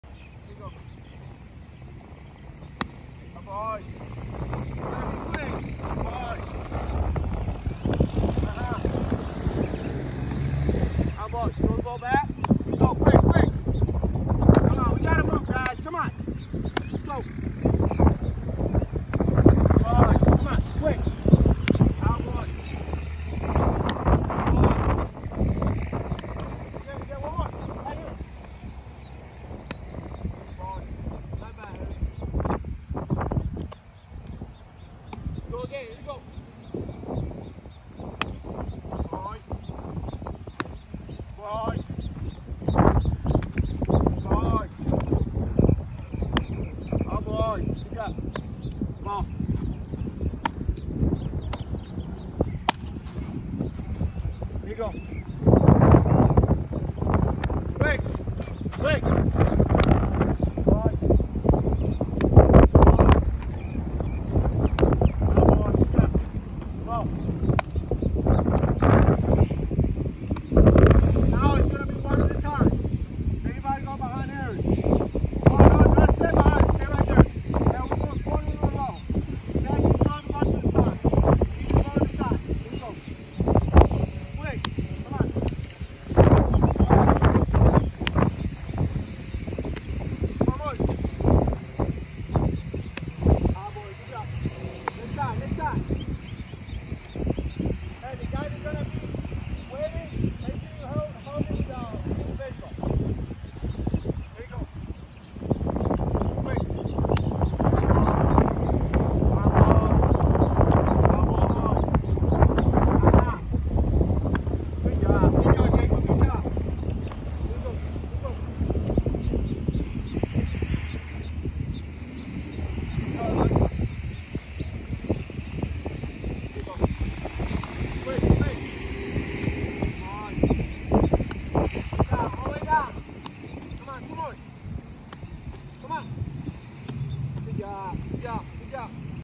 Catcher's Group Receive and Pop Drill
Elementary school catchers practice at a catching group with Johan Limonta and Aberto Cordero. They drill in a line receiving pitches from Limonta and quickly transitioning to the throwing position.

Sugar Land Skeeters' assistant coach Johan Limonta and player Alberto Cordero run youth hitting and catching groups in Katy, Texas.